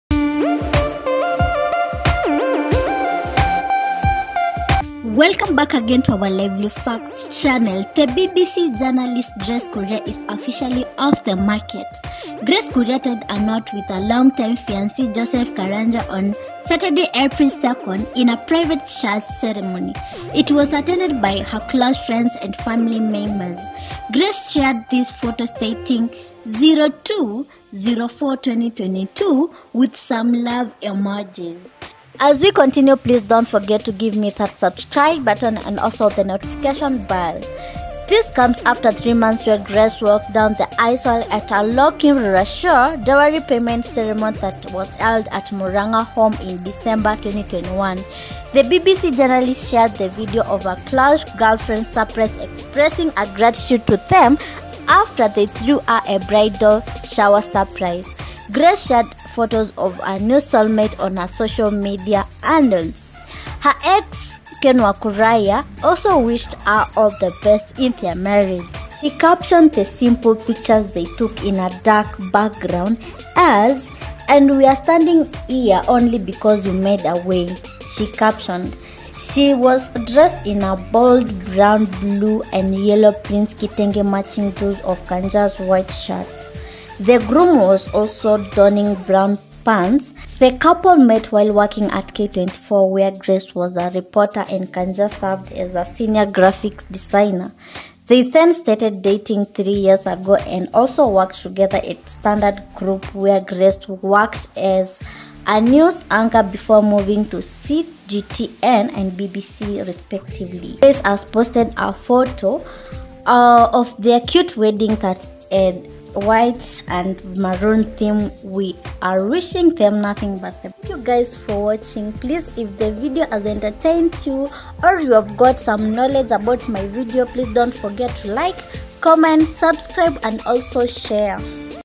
BBC journalist Grace Kuria Lavish private Wedding.CONGRATULATIONS
BBC journalist Grace Kuria is is officially off the market. Grace Kuria tied a knot with her longtime fiance Joseph Karanja.on sat april second in a private church celeremony .It was attended by close friends and family members. Grace share this photo stating 02.04 trandingno1.